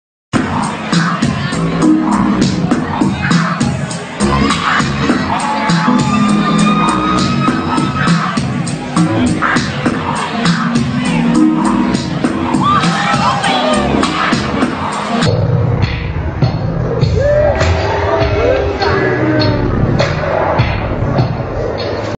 See's What Happens When A Stripper Pole Snaps Into Two (lol)
Stripper Eats It Hard When Pole Snaps Into Two